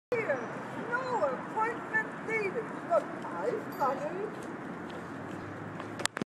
Flu Shots. No appt needed...
Petra liked this
She thought it was funny
Flu shots on the street for free...
Hmm...